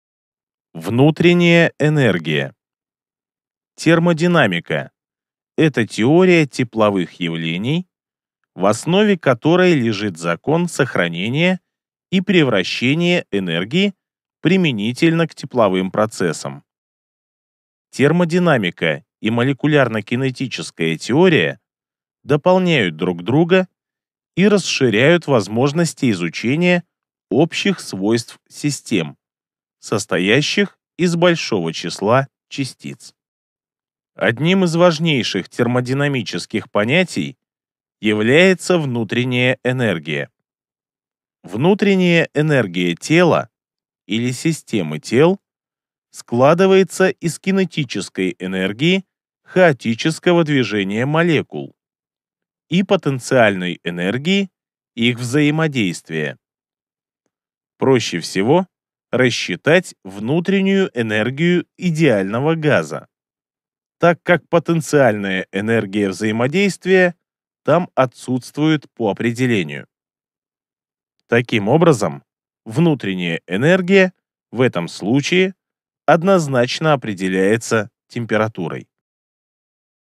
2.2.1 Внутренняя энергия. Термодинамика
0:04 - Термодинамика - это теория тепловых явлений, в основе которой лежит закон сохранения и превращения энергии.
0:34 - Внутренняя энергия или системы тел складывается из кинетической энергии хаотического движения молекул и потенциальной энергии их взаимодействия.
Идется о термодинамике, подаются формулы